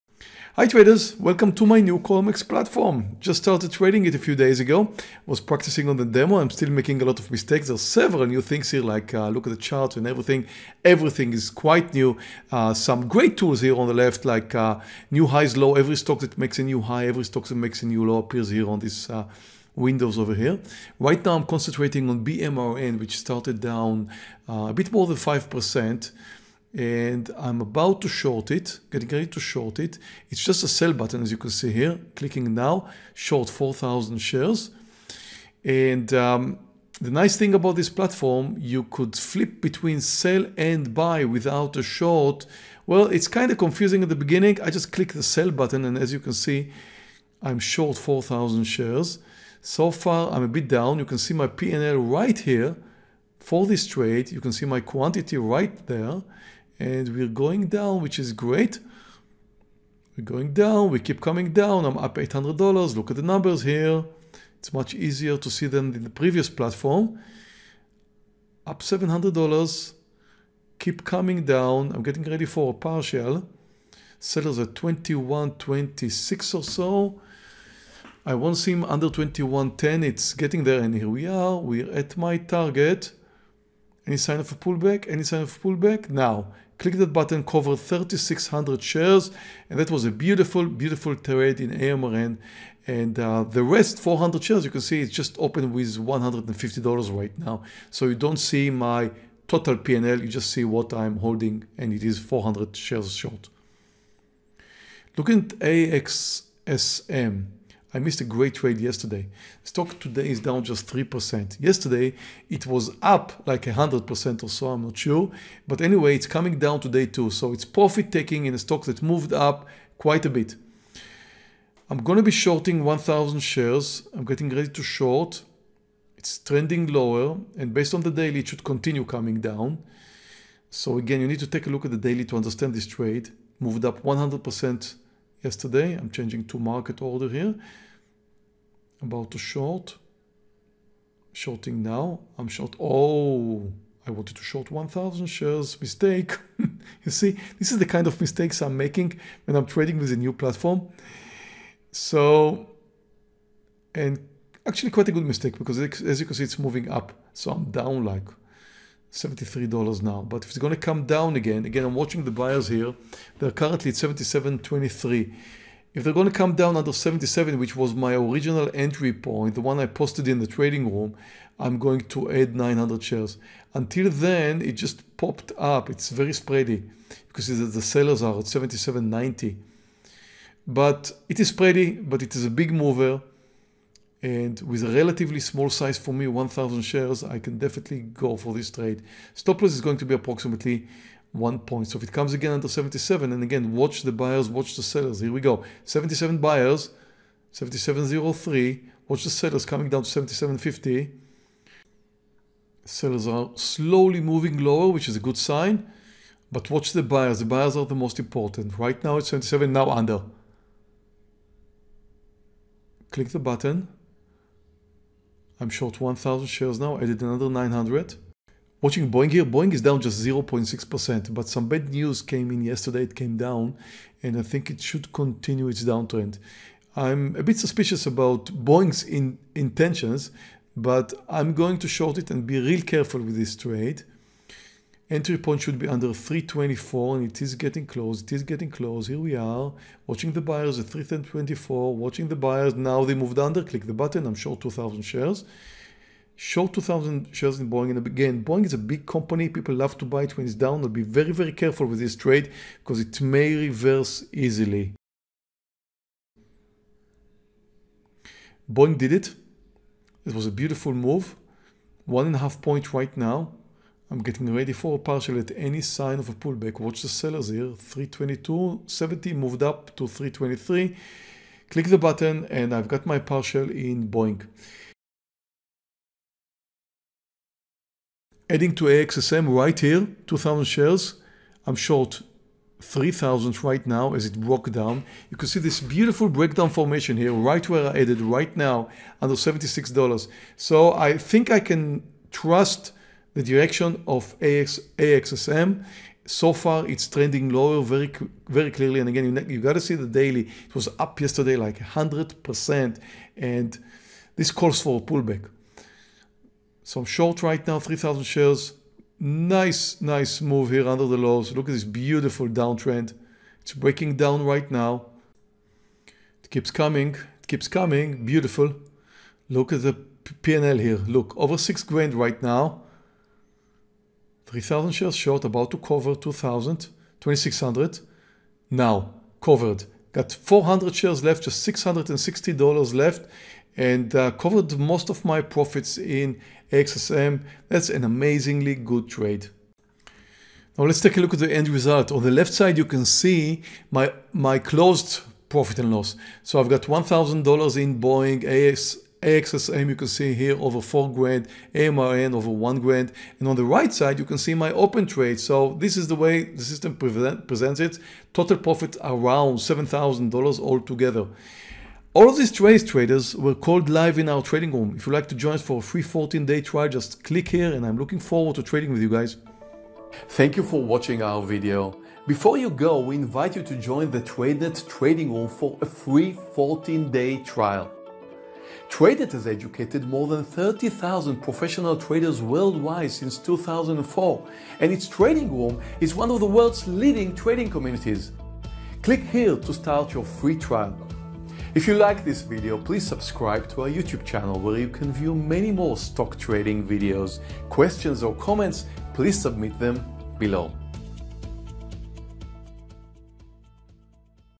Live Day Trading AXSM for $7,000
Live Day Trading Stocks - How to make money by day trading stocks -  Shorting AXSM for $7,000 in profits. Meir Barak day trading live on December 17th. 2019.

🎅⛄ Exclusive Christmas Discounts Across All Tradenet Funded Account Programs!
(Valid: Dec 16 - 26)